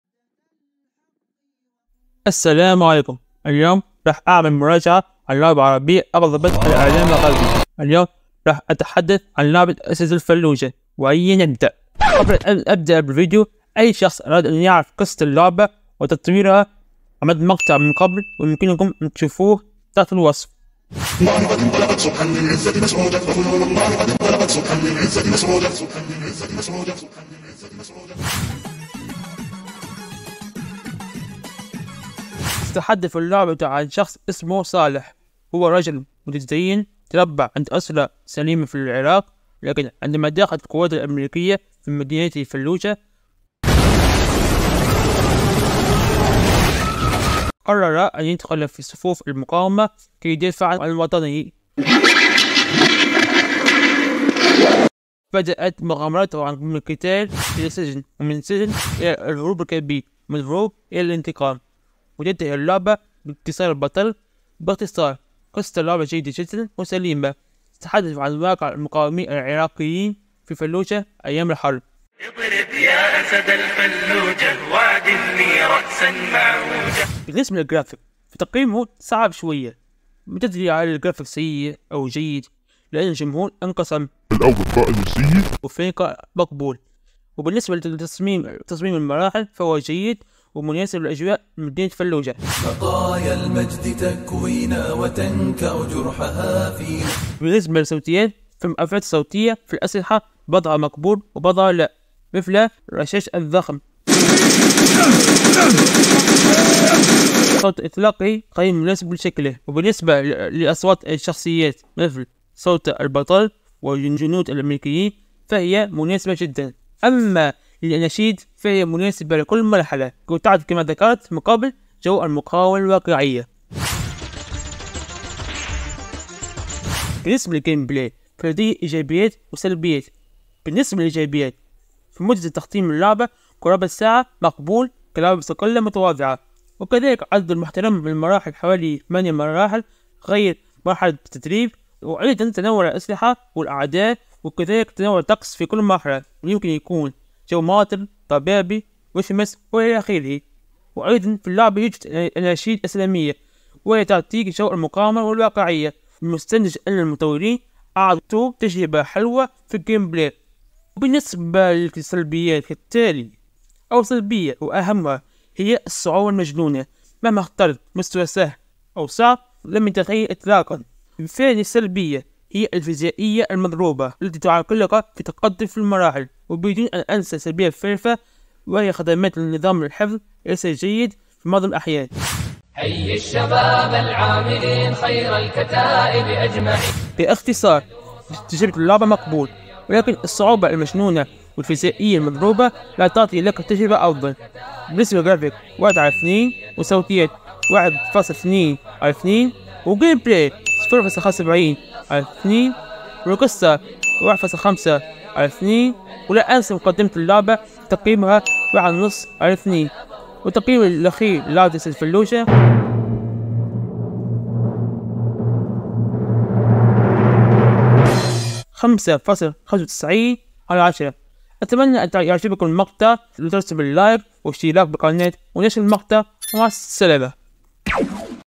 اللعبة التي أغضبت الاعلام الغربي , مراجعة لعبة أسد الفلوجة
رابط قصة مقطع لعبة أسد الفلوجة